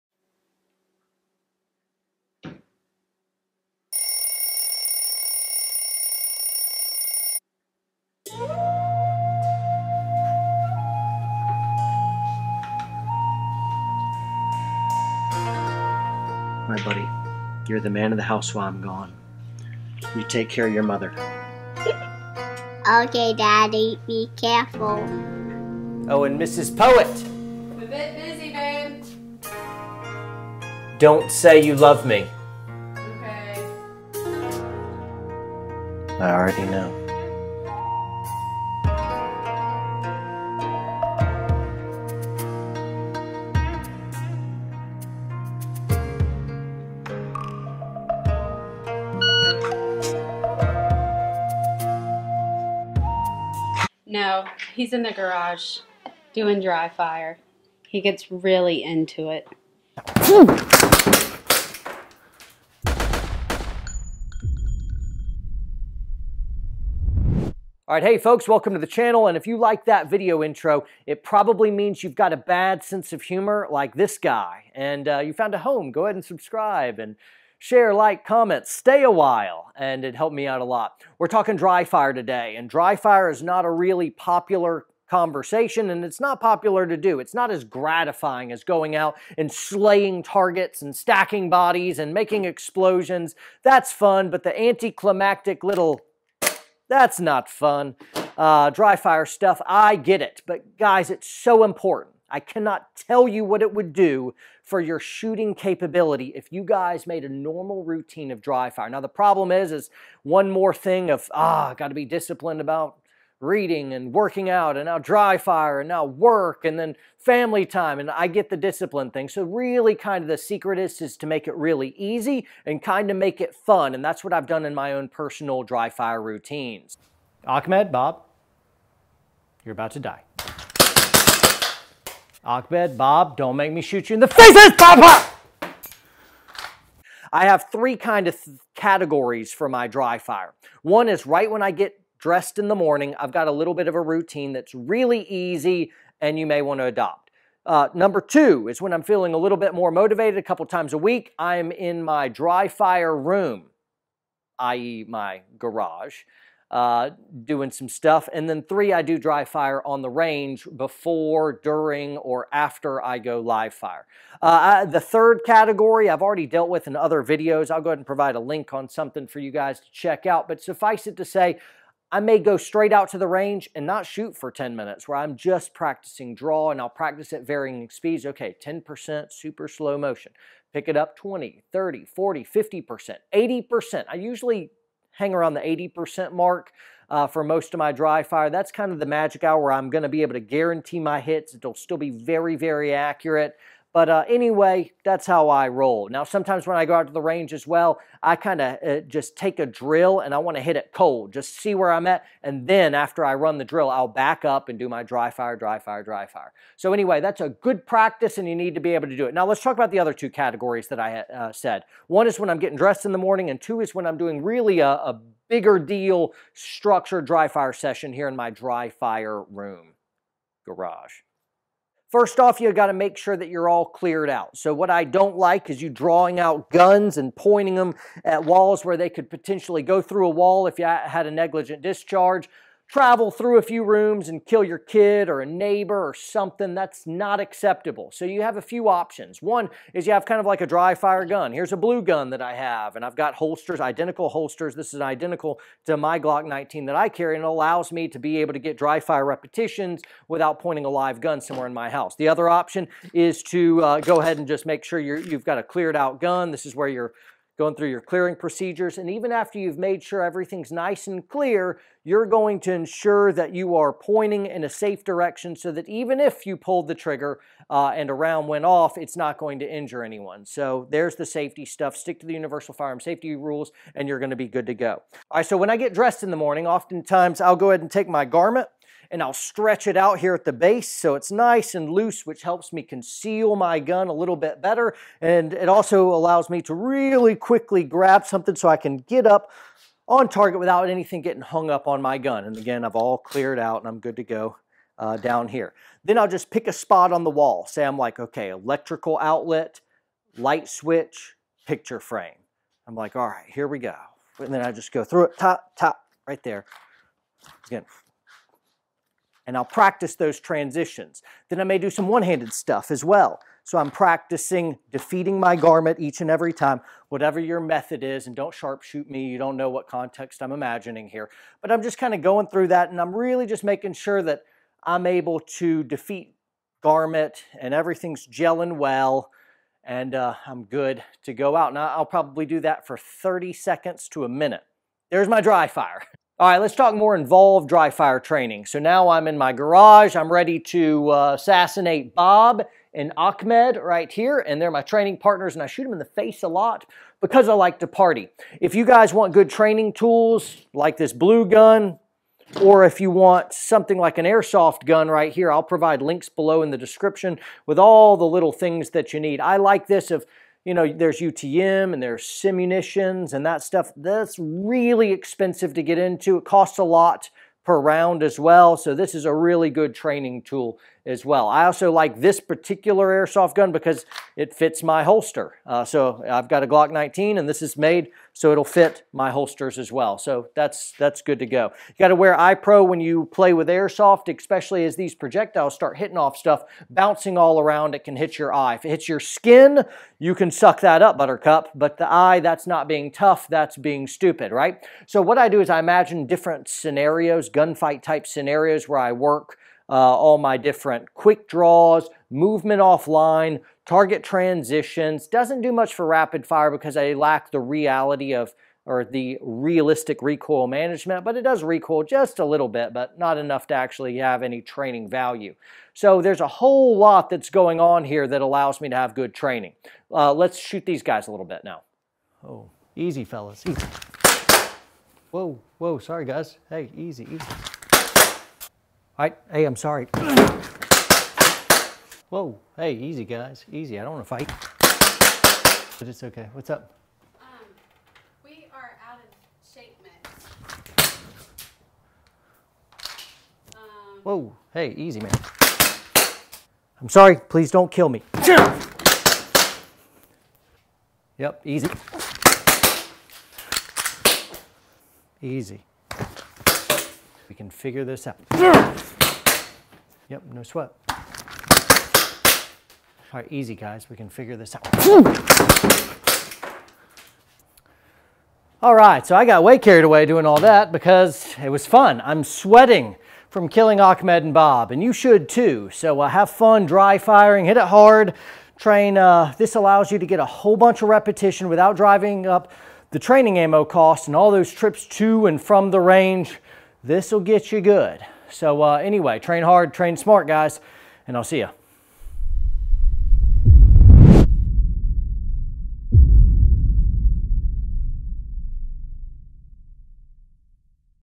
How to Dry Fire + funny intro
Dry-fire training is absolutely critical if you want to excel as a shooter. The good news is that it is very cheap, easy to do anywhere, and pays huge dividends. The bad news is it is not as fun and falls into that category of 'discipline'. Here is how I spice things up so my dry fire practice is easy, FUN, and incredibly helpful.

RECOMMENDED GEAR

Stuff in Video:
-Airsoft gun
-Gunbelt (see 'Recommended Gear' above)

Recommended Gear